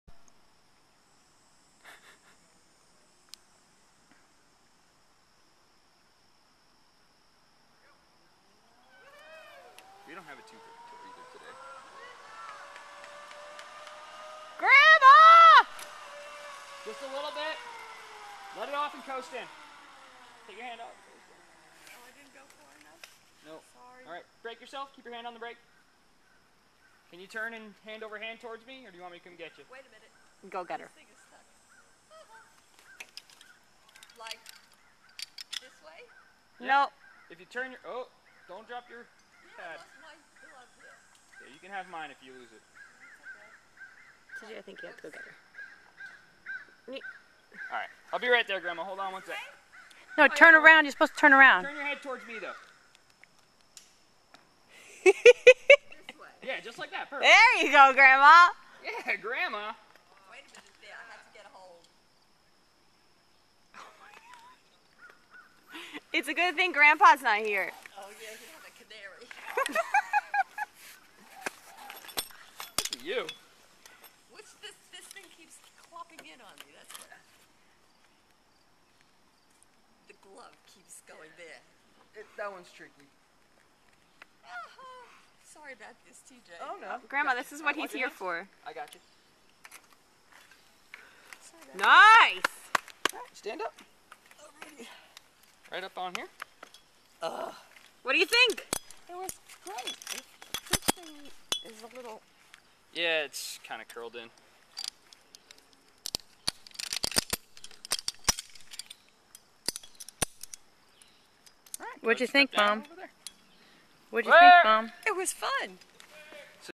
This is How my Mom rolls !
Zip-lining through the treetops at Morningside Flight Park